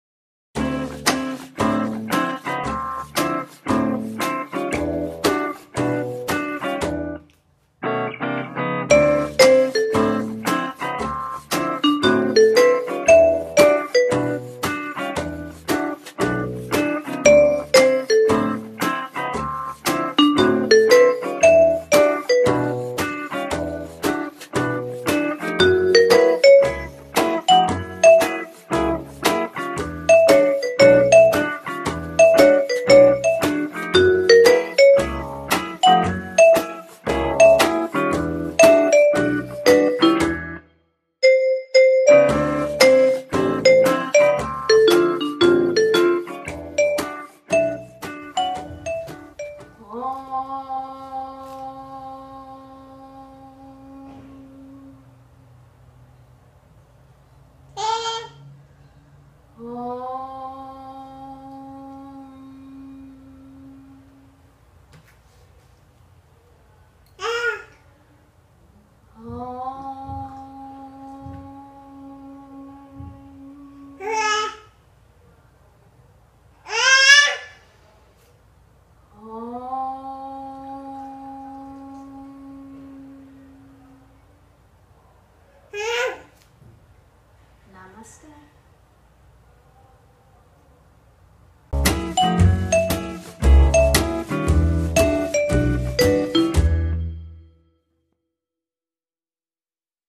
Yoga on a Crowded Mat
A yoga practice with a mom, 8 month old baby, and a rhodesian ridgeback.